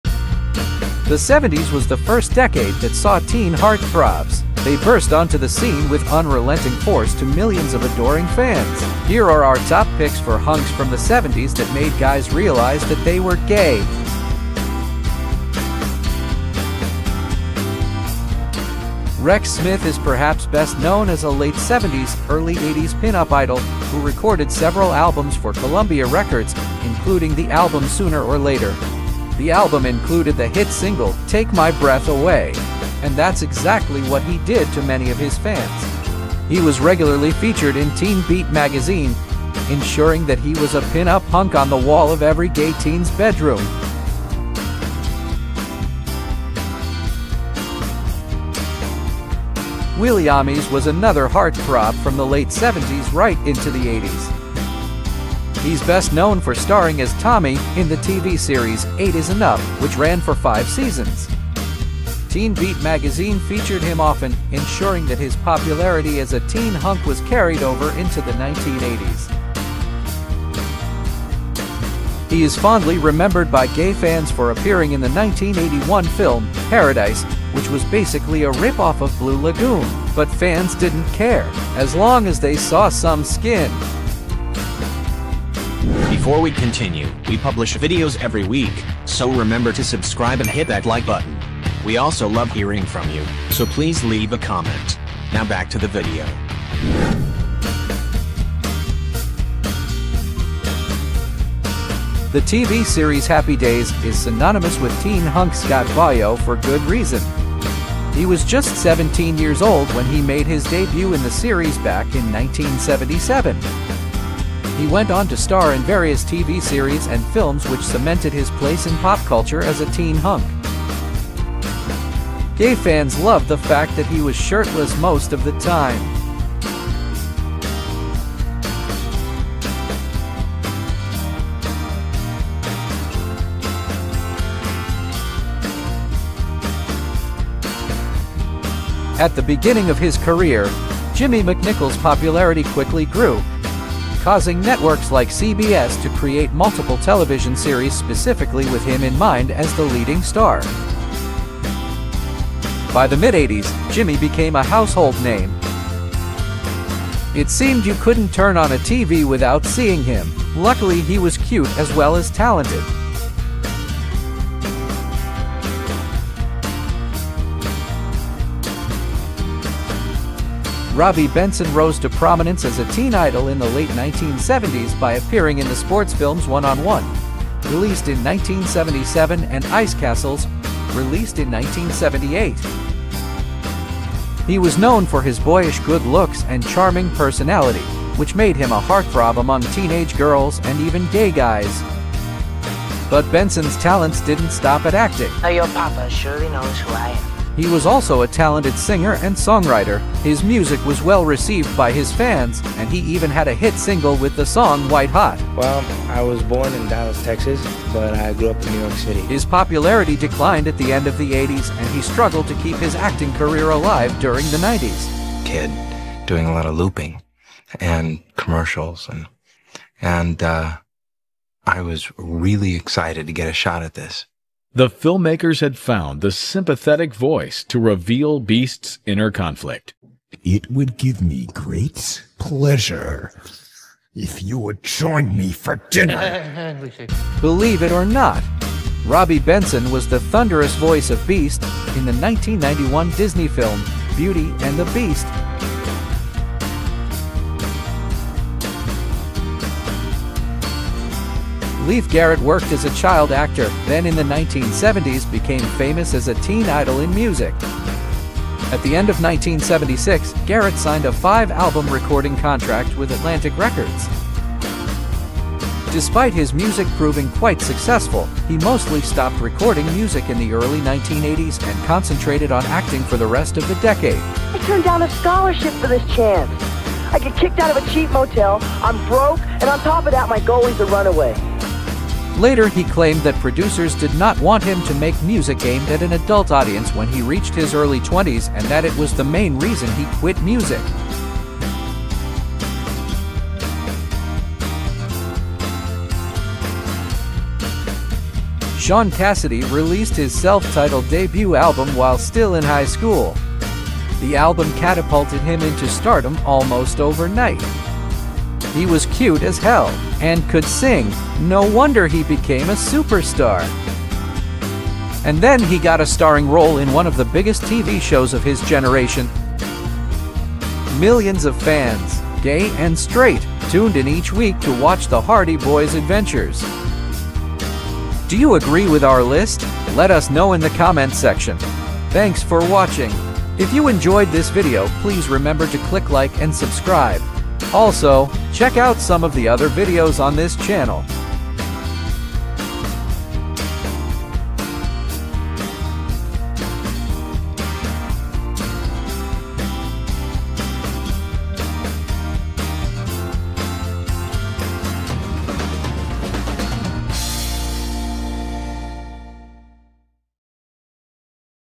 70s pinup stars who HELPED guys realize they are GAY
The 70s was the first decade to embrace the idea of teen hunks and pinup idols. Dozens of new faces got thrusted into the limelight, much to the delight of gay fans everywhere.

Here are our top picks for 70s pinup hunks who made guys realise that they were gay.

00:00 Intro
00:14 Rex Smith
00:45 Willie Aames
01:37 Scott Baio
02:14 Jimmy McNichol
02:46 Robbie Benson
04:07 Leif Garrett
04:54 Shaun Cassidy